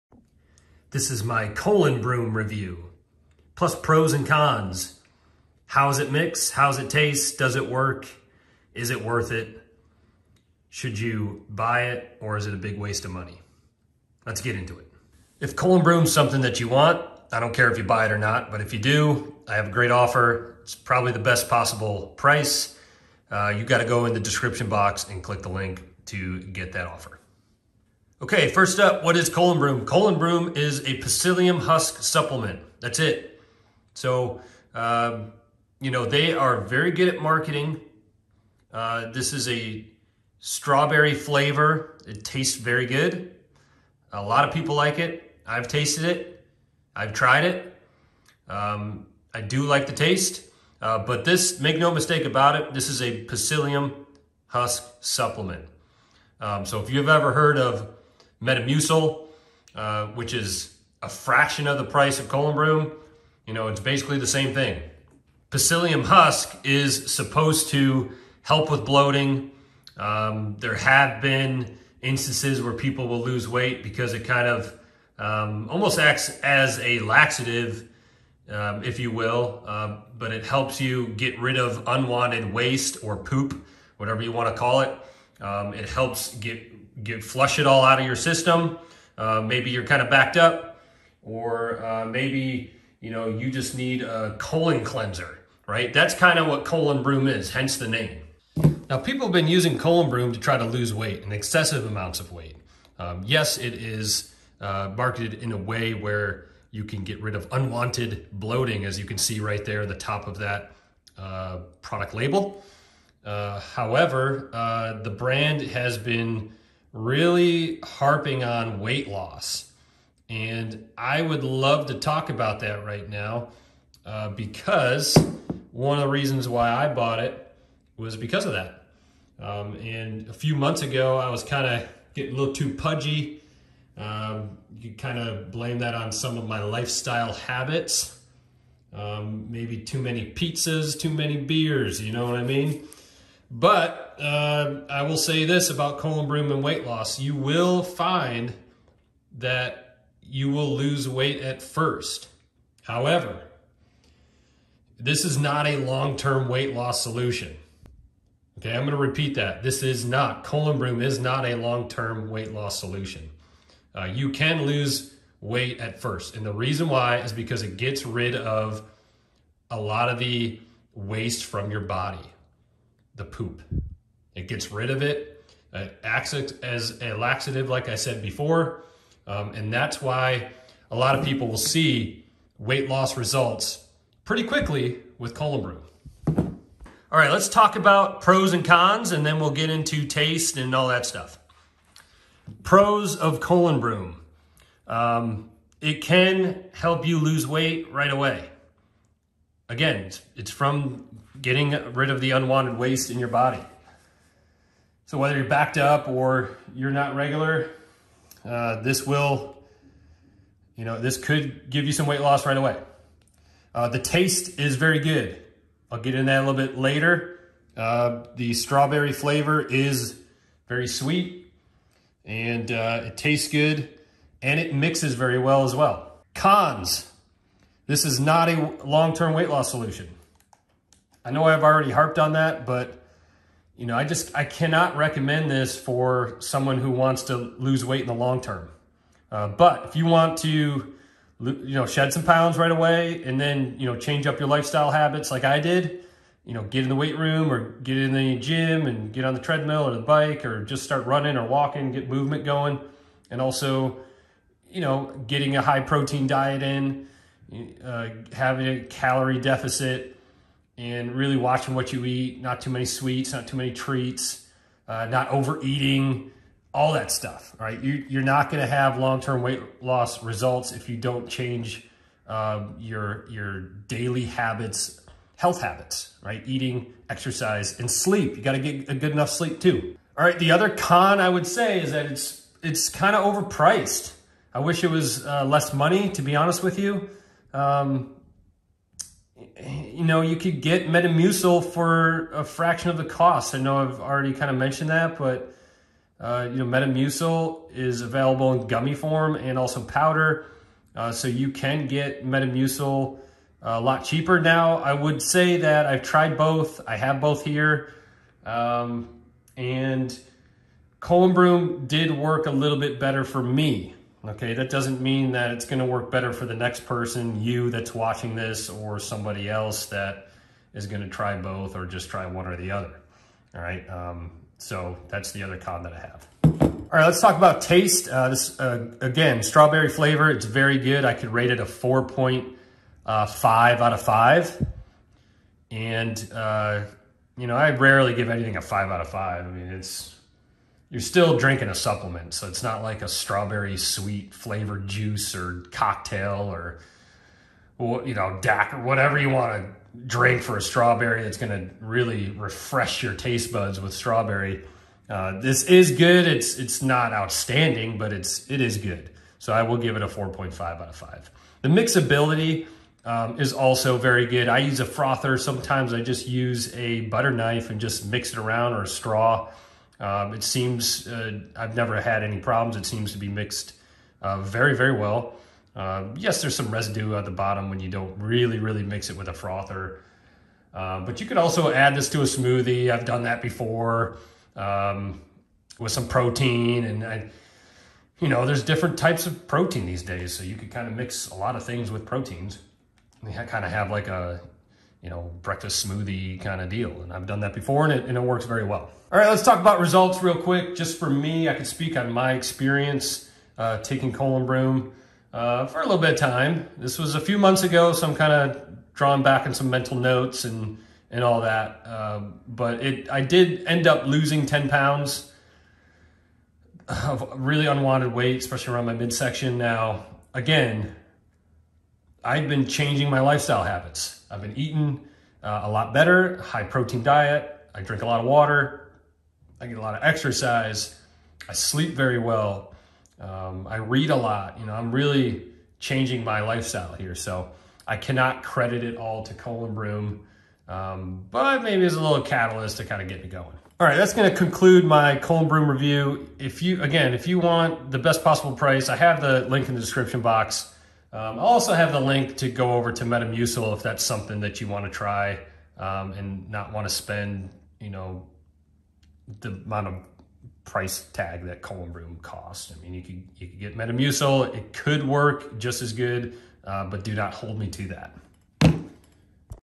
Colon Broom Review - Pros, Cons, Did It Help Me Lose Weight?
This is my ColonBroom review and experience taking it. I wanted to shed 10 pounds, tried this and here's what I think. I'll also provide my pros and cons and taste rating.